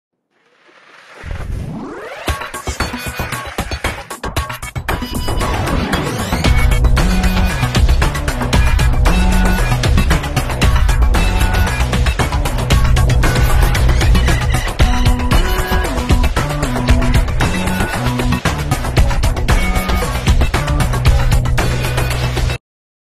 EEC L2e Electric Tricycle 3 wheel electric vehicle
EEC L2e electric tricycle for sale, looking for dealers around the
world.

Electric vehicle approved by EEC COC DOT.
64V 105AH  Lithium iron phosphate battery
3KW/4KW motor
Max.speed: 45Km/h
Max.range: 100-120Km
WhatsApp/WeChat: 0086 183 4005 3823

For more details, please contact with Cindy Yang

Vic